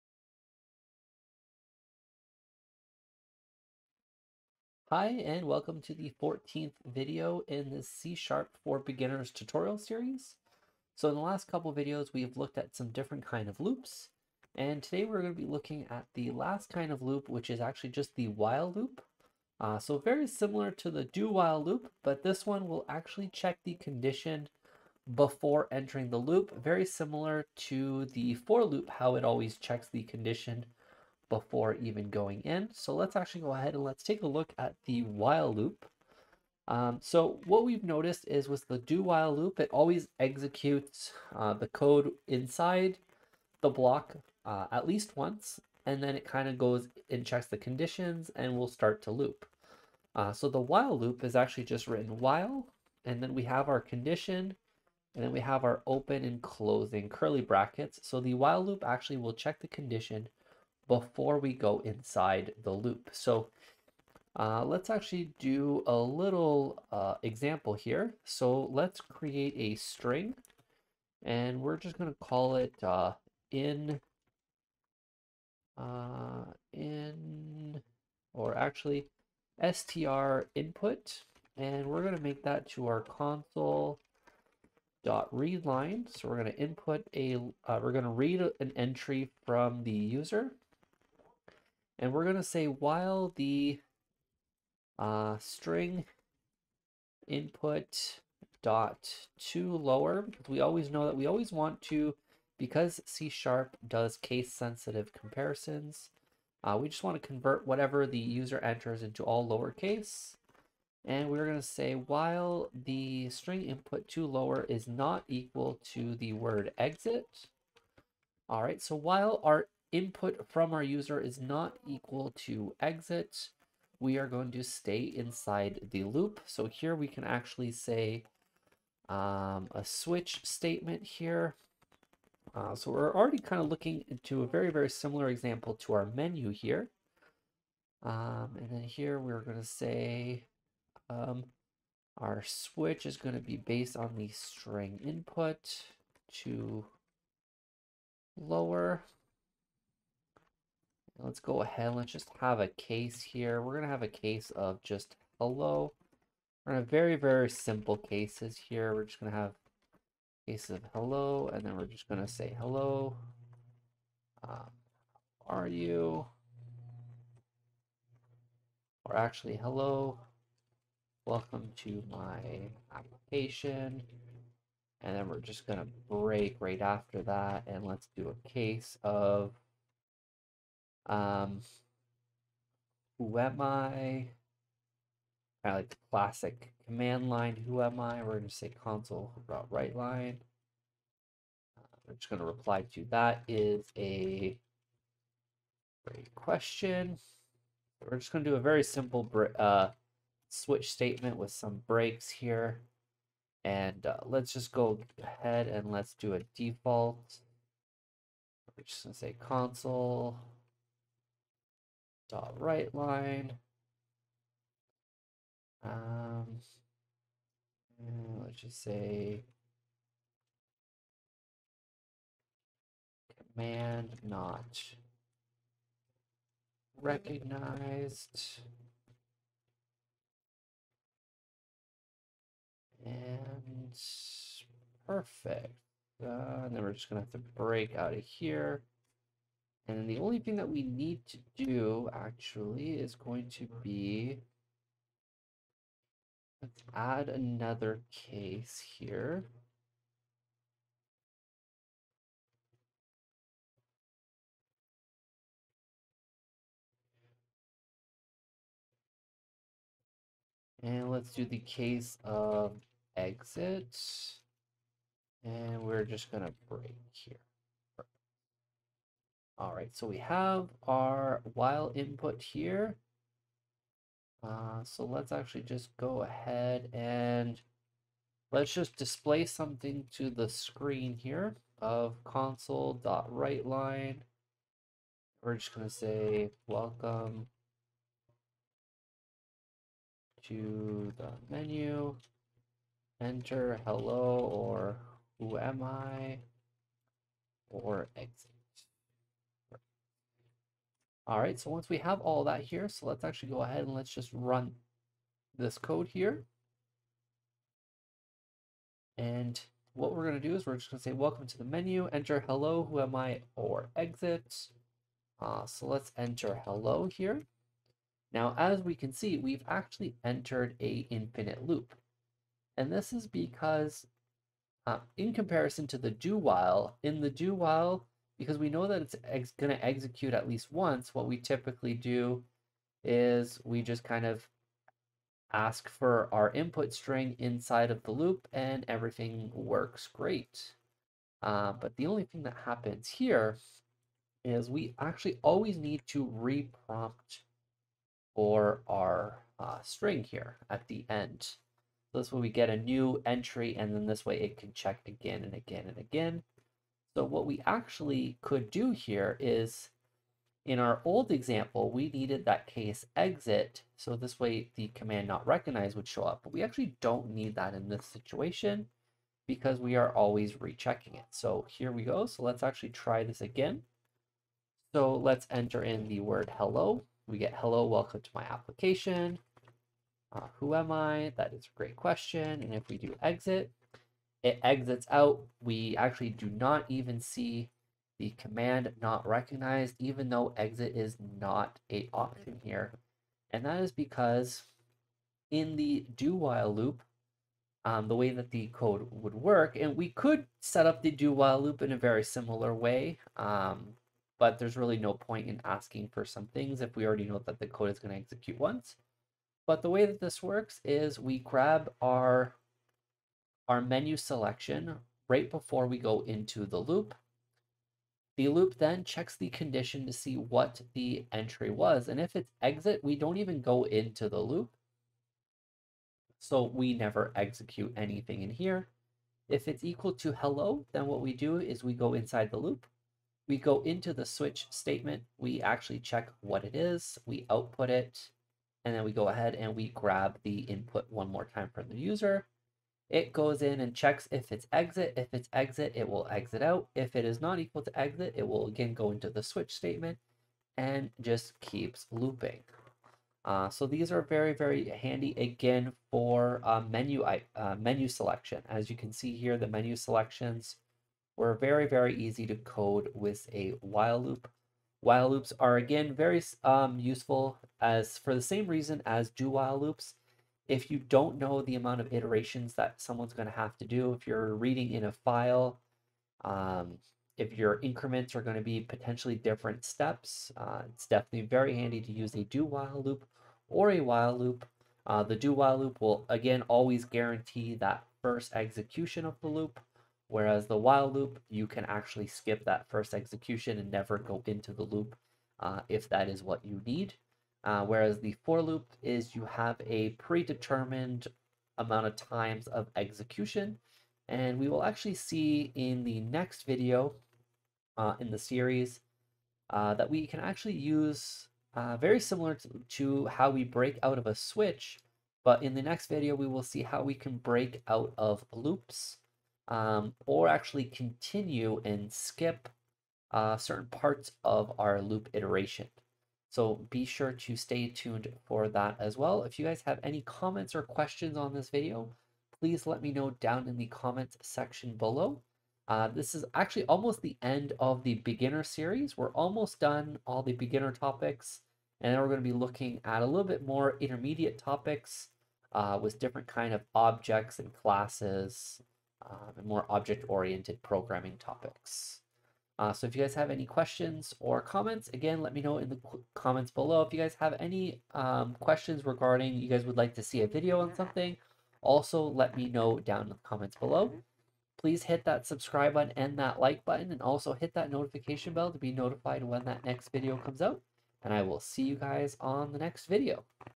C# Tutorial for Beginners 14 : While Loops
In this series we will learn C# from the basics to advanced topics. In this video we will see how to use the while loops in C# and see a use case scenario for them and discuss the overall differences between the for, do while and while loops